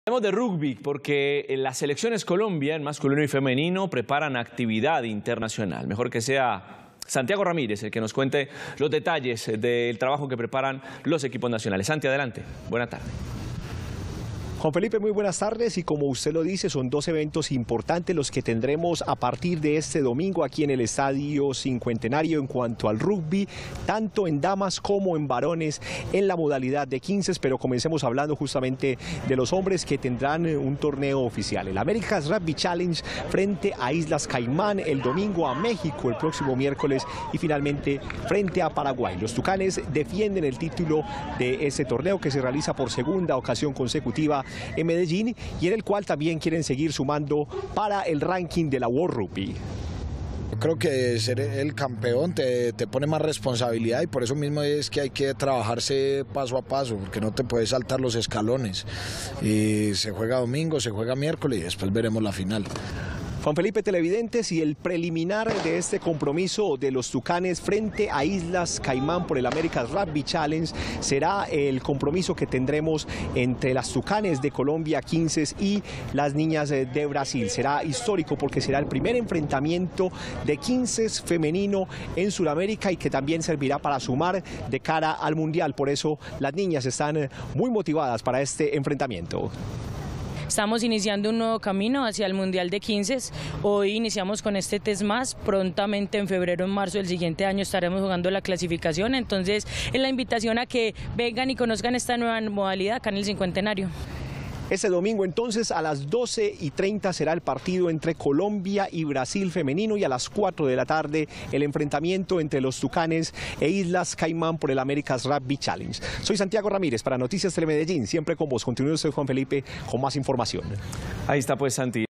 Selecciones Colombia de Rugby entrenarán en el Estadio Cincuentenario [Noticias] - Telemedellín
Al medio día las Selecciones Colombia de Rugby que participarán del Americas Rugby Challenge y el Test Match Femenino realizarán el primer entrenamiento oficial en el Estadio Cincuentenario.